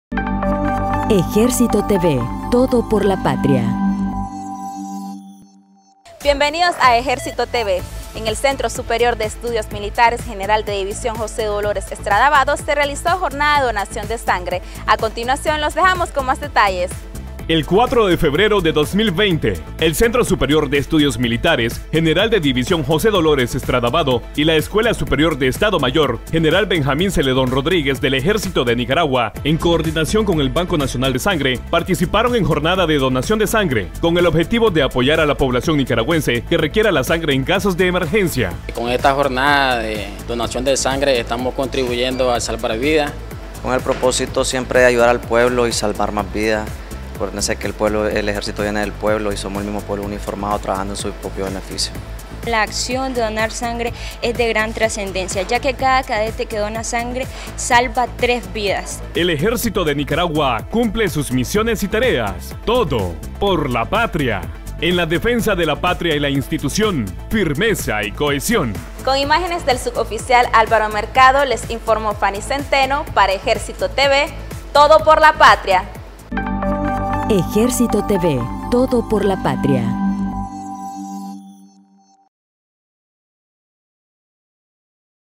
CSEM y ESEM participaron en jornada de donación de sangre
El 4 de febrero de 2020, el Centro Superior de Estudios Militares “General de División José Dolores Estrada Vado” y la Escuela Superior de Estado Mayor “General Benjamín Zeledón Rodríguez” del Ejército de Nicaragua, en coordinación con el Banco Nacional de Sangre, participaron en jornada de donación de sangre con el objetivo de apoyar a la población nicaragüense que la requiera en casos de emergencia.

Ejército TV